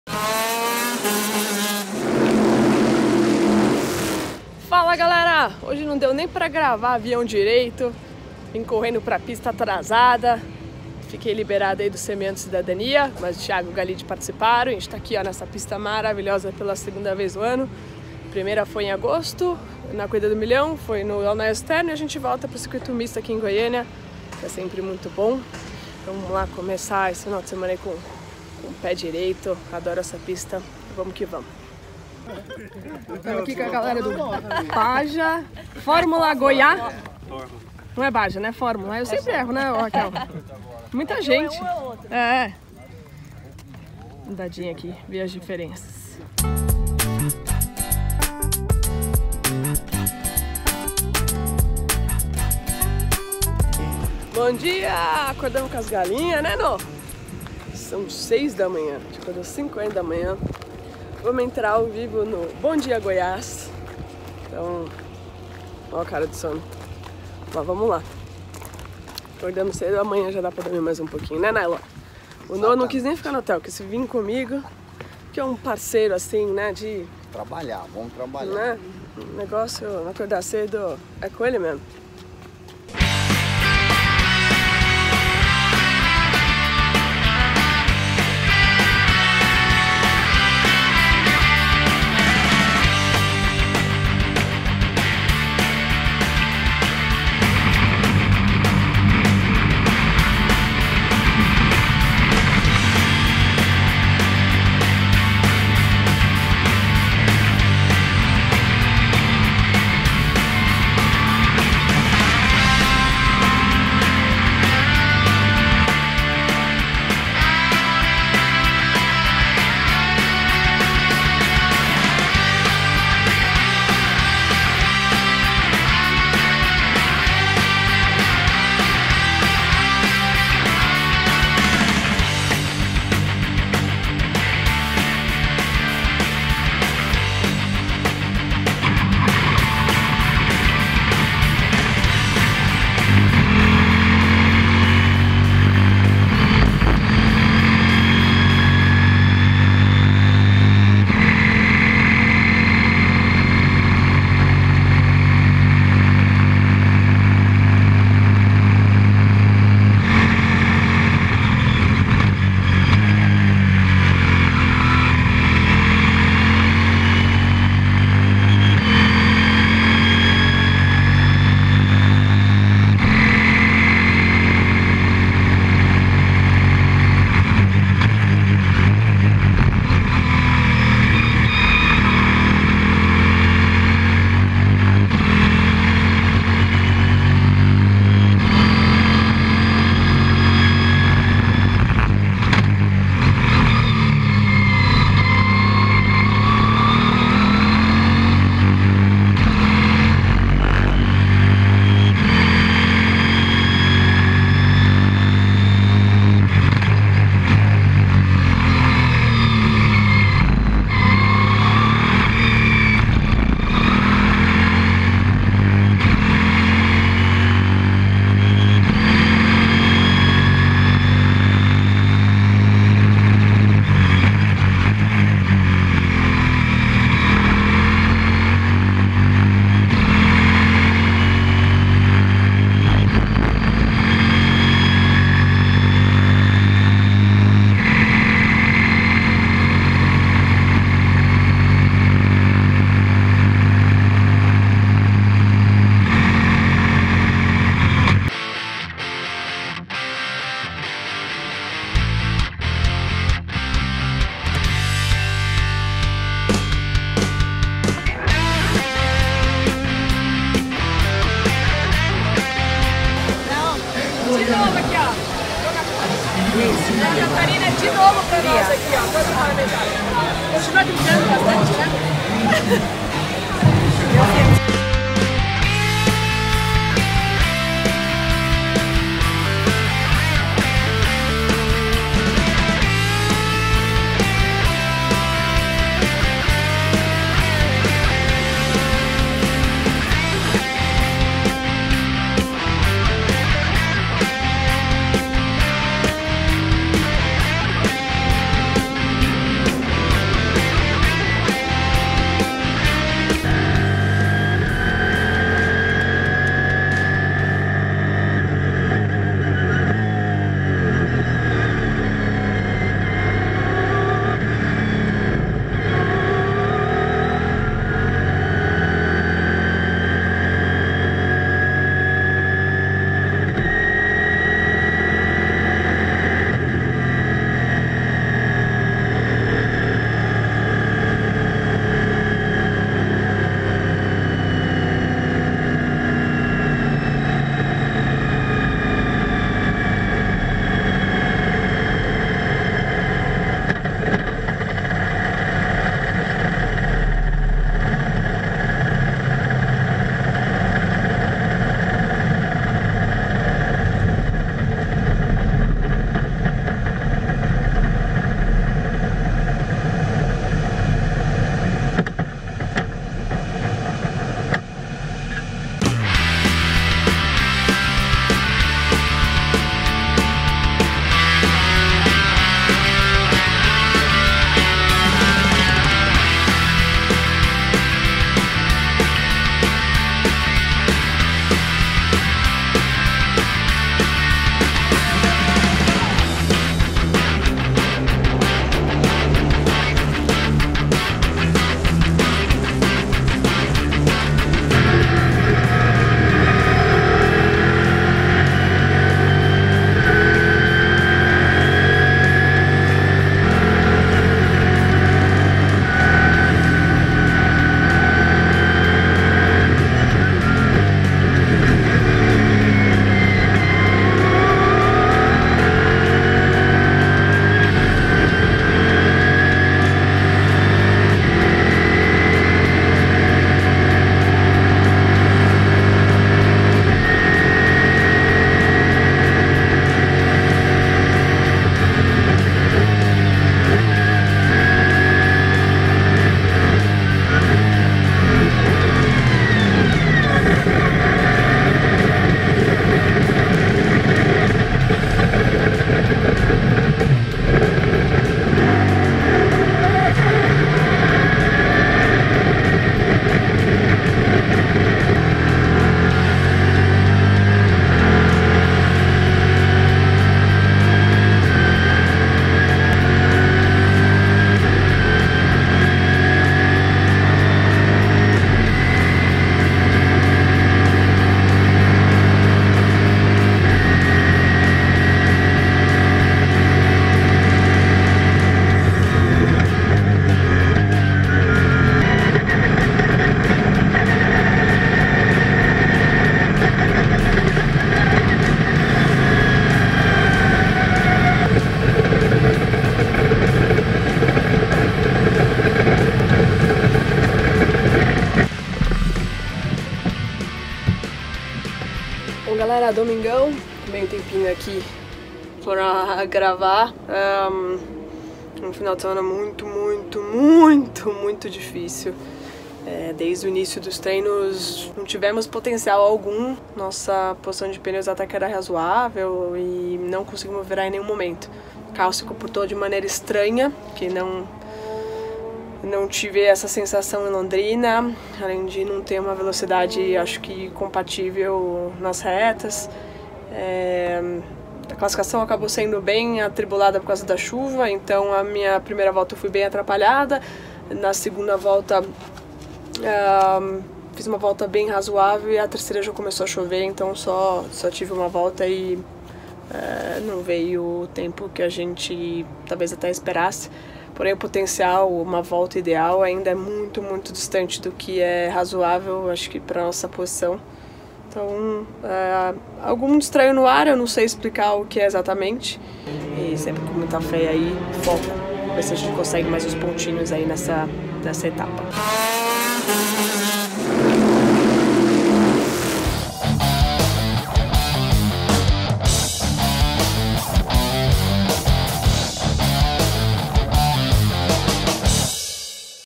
11ª Etapa da Stock Car em Goiânia
Confira como foi os bastidores da penúltima etapa do campeonato.
A próxima etapa, acontece em São Paulo no autódromo de Interlagos, e terá pontuação dobrada.

Fotos: Hyset
Imagens  Vicar
Edição - Ale Jardim
Arte - Raphael Luz
Capa: Guilherme Gaino